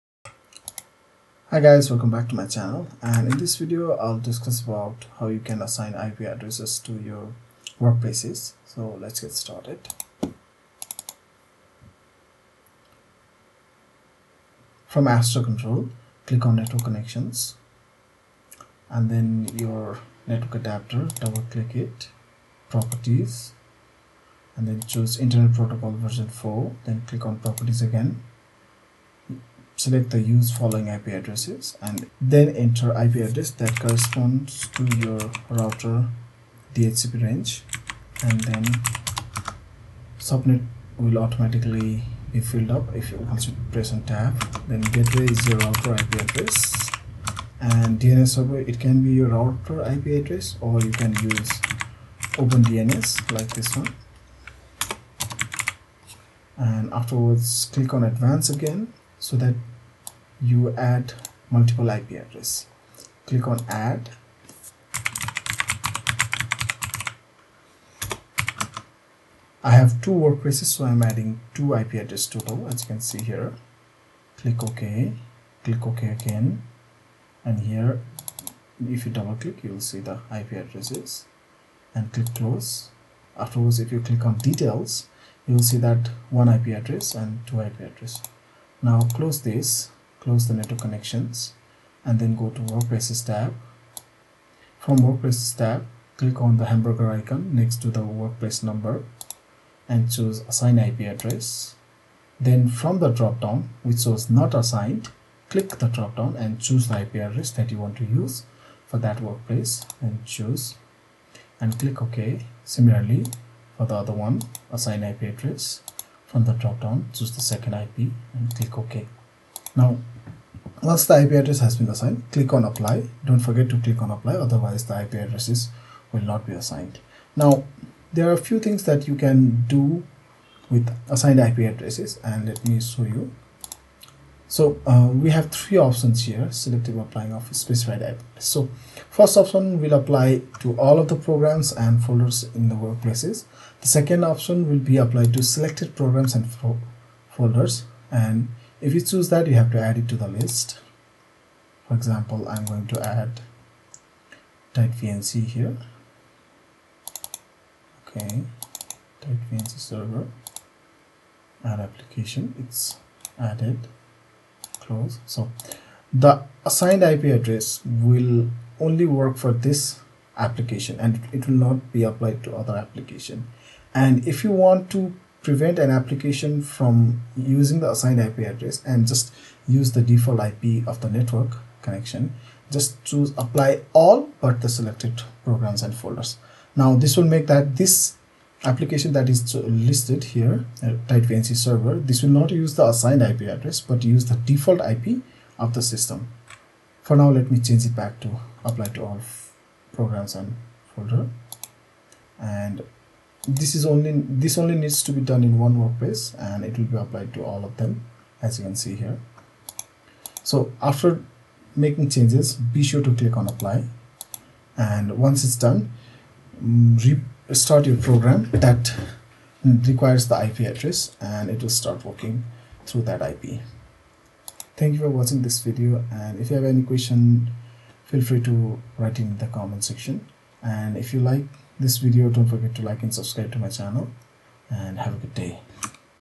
How to assign IP address to ASTER workplaces
In this video, I will show you how to set Static IP addresses to your network adapter and then assign those IP to workplaces from ASTER control.